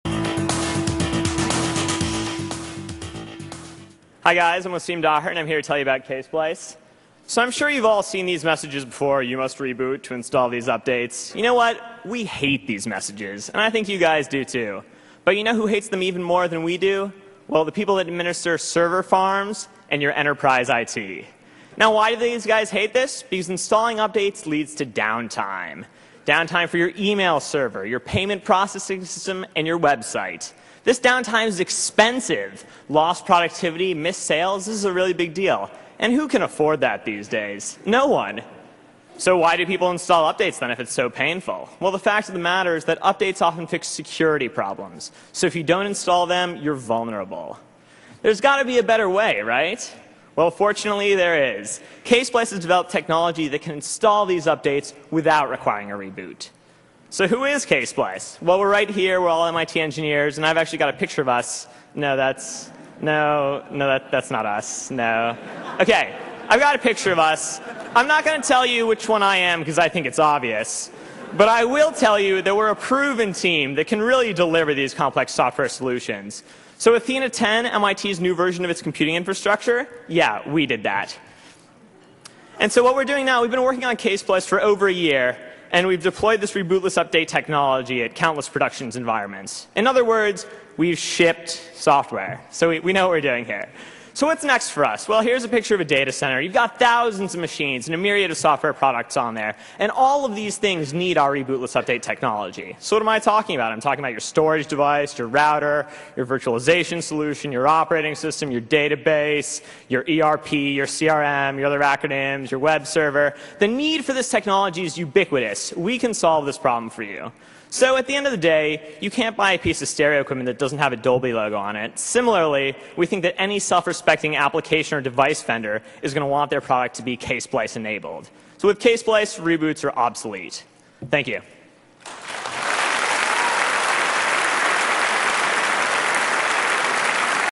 Ksplice at MIT $100K Business Plan Contest
This is a clip from the MIT $100K Business Plan Contest, where Ksplice won the Grand Prize.